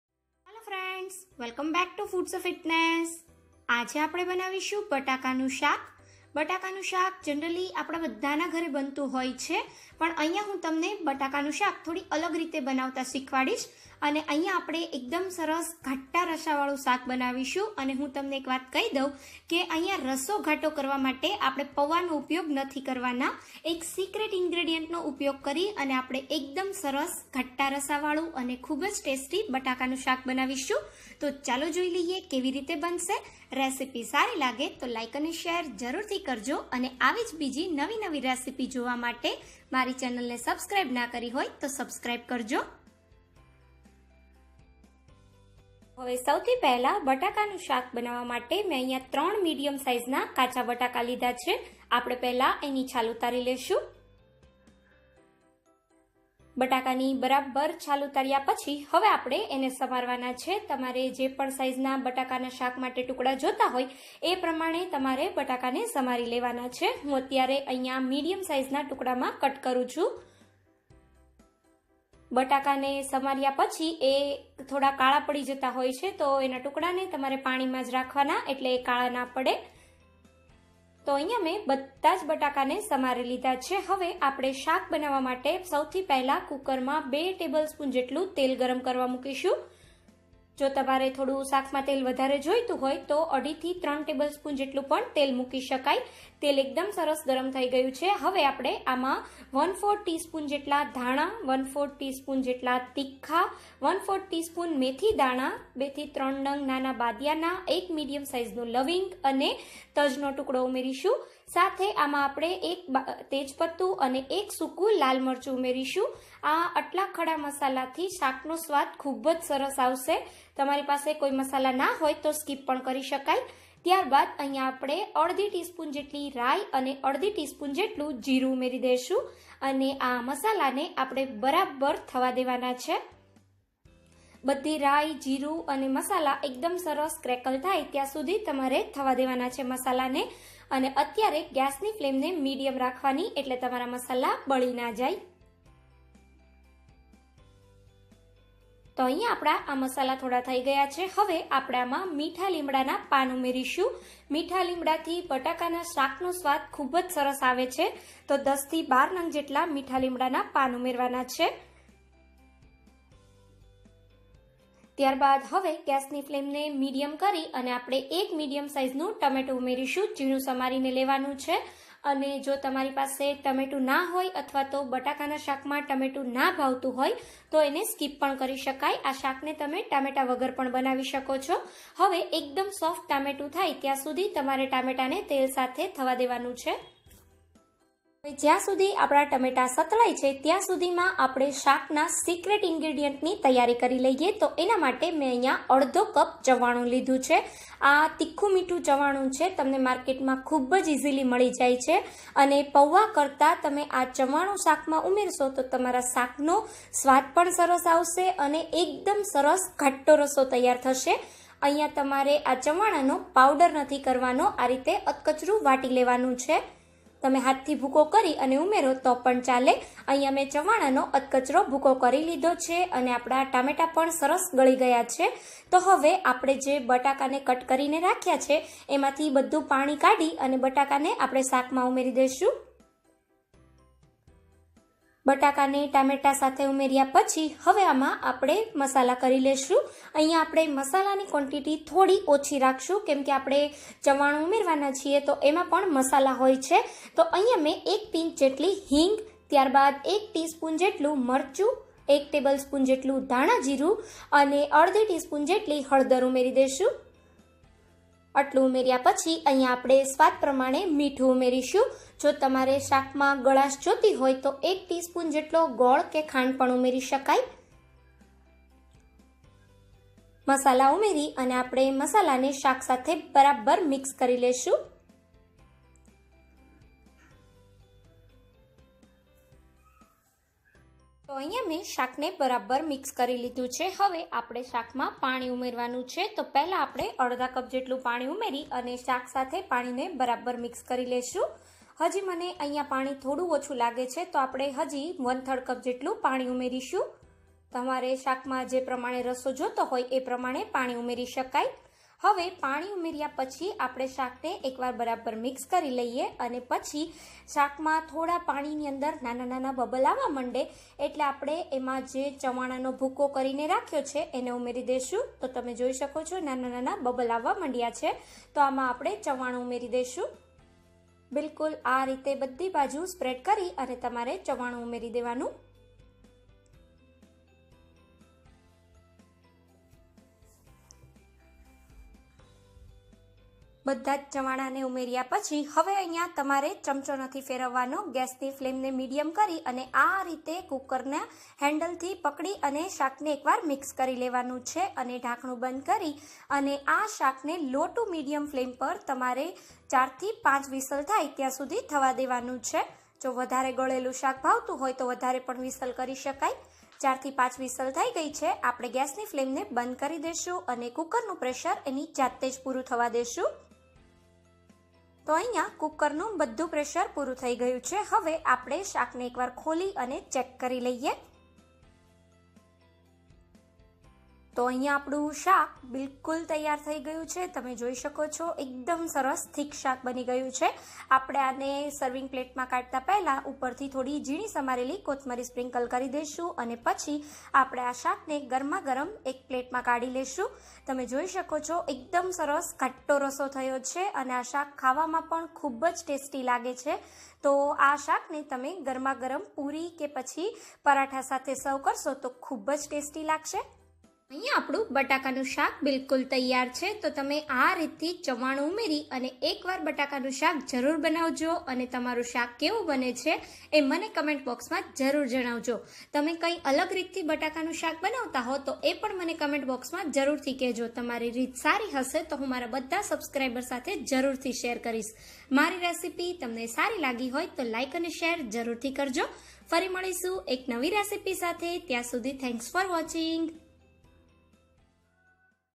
તહેવારોમાં એકદમ નવી રીતે ટેસ્ટી અને ચટાકેદાર રસાવાળા બટાકા નું શાક બનાવાની રીત/Bataka nu Shaak
અને સાથે બેલ બટન પર ક્લિક કરી દો જેથી નવી નવી રેસિપી ના વિડિઓ